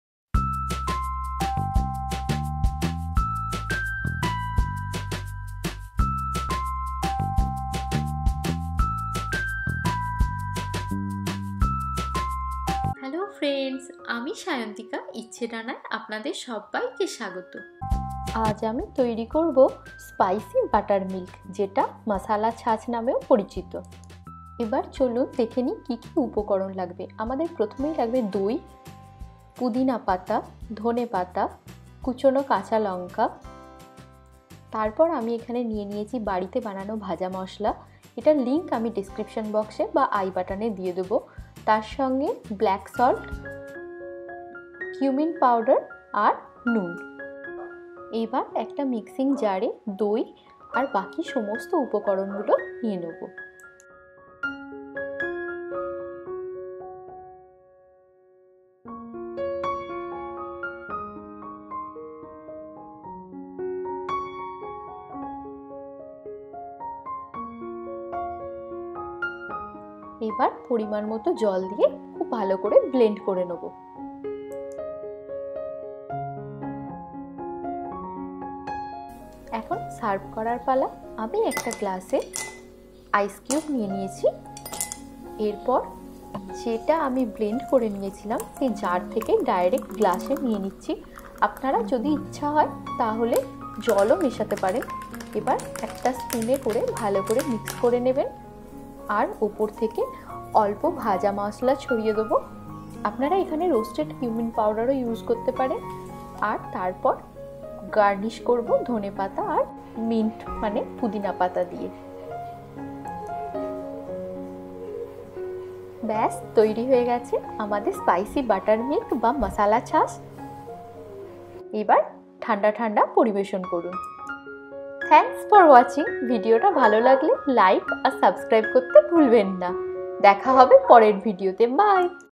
Masala Chaas Recipe| Masala Taak - Indian Summer Drink | How To Make Masala Chaas
Learn how to make masala chaas(spicy butter milk) at home with Sayantika only on Icche Dana.